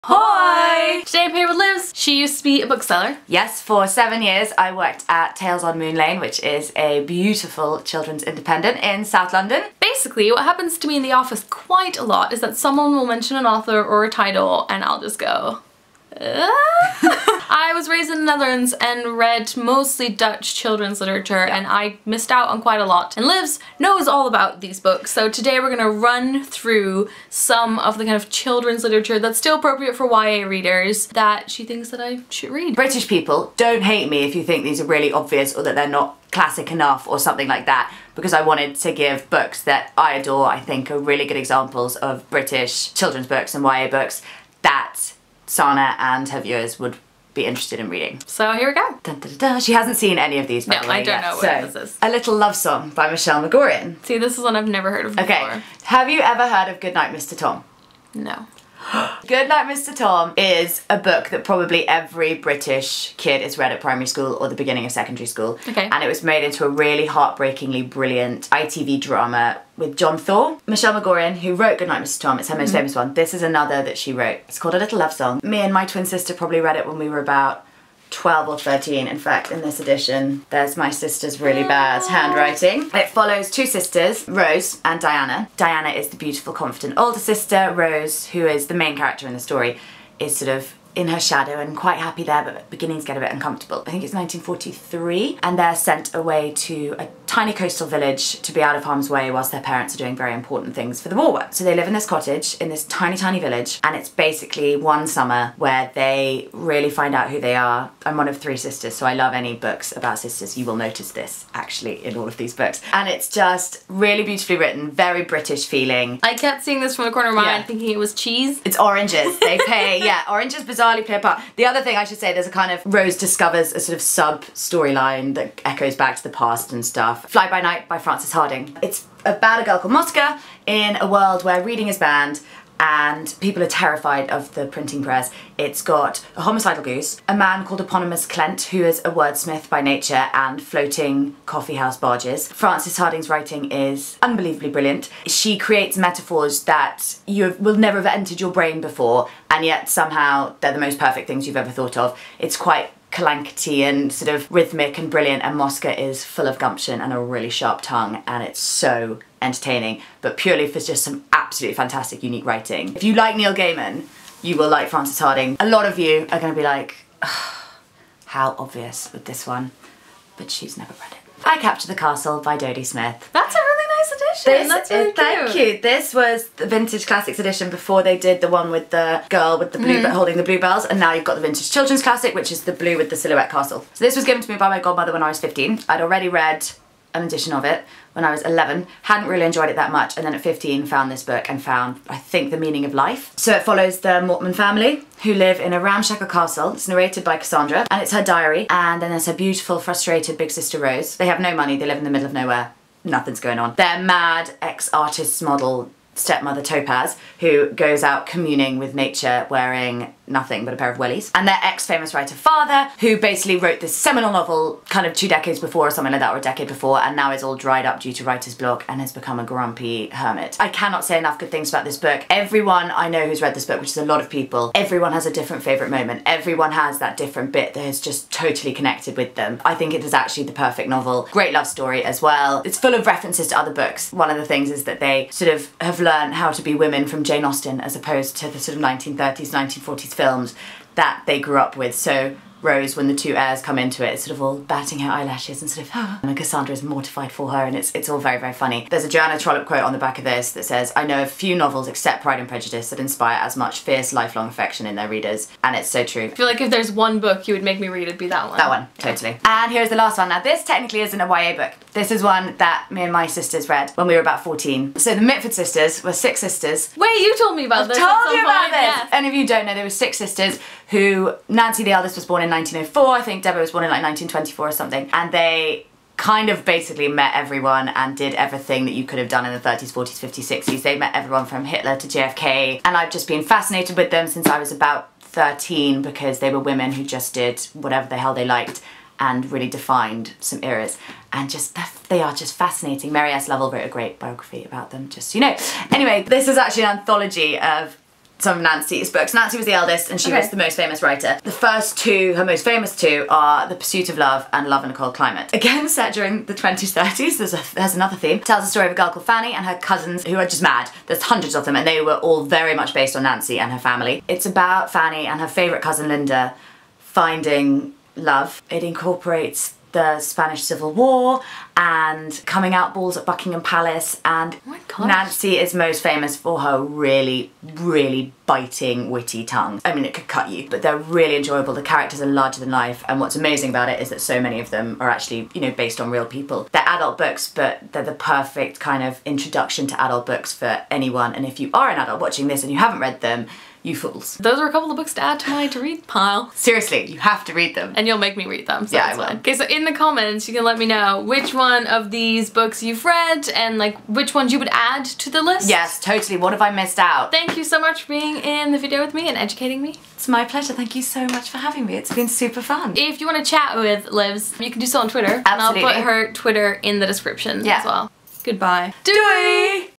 British YA Classics with Livs.
Or: books I missed out on by not growing up in the UK.

(Also, YA didn't exist at the time that these books we're written, but Livs selected books she thought would be great for YA readers.)

This was recorded quite a while ago and I have since bought I Capture the Castle. I've promised Livs that we'll do a double review when I've finished it.

Livs on Twitter.

Tales on Moon Lane.
25 Half Moon Lane,
London, SE24 9JU

Books mentioned in this video:

A Little Love Song by Michelle Magorian.

Fly by Night by Frances Hardinge.

I Capture the Castle by Dodie Smith.

Love in a Cold Climate & The Pursuit of Love by Nancy Mitford

Follow what I'm reading.